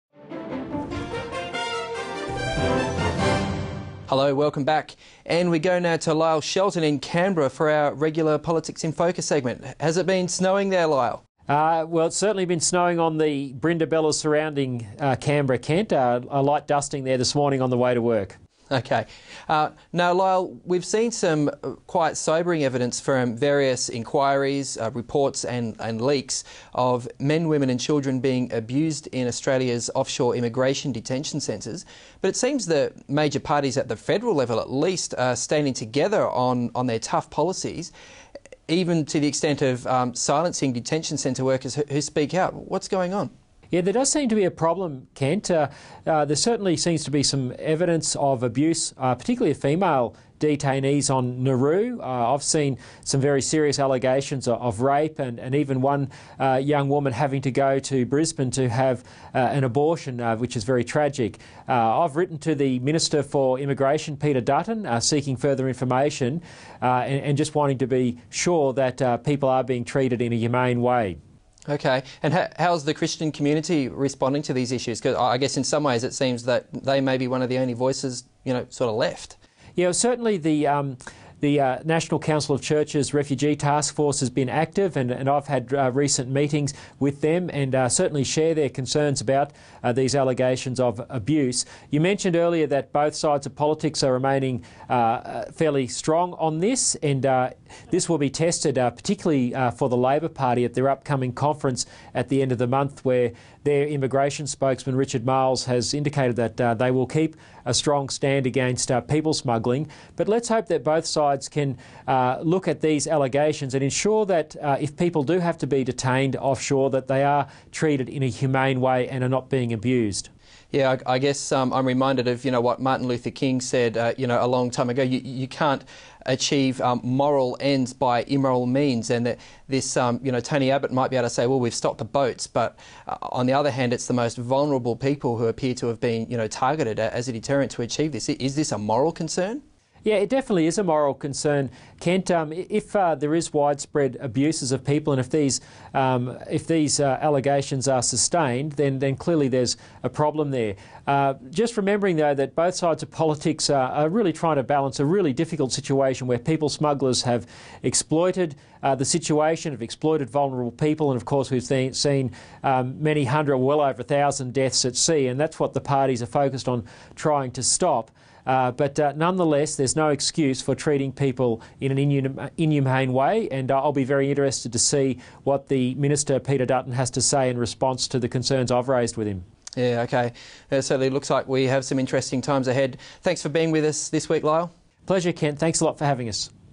Asylum seekers: a moral issue
The Australian Christian Lobby's Lyle Shelton is concerned at the lack of compassion in the policies of both of Australia's major parties.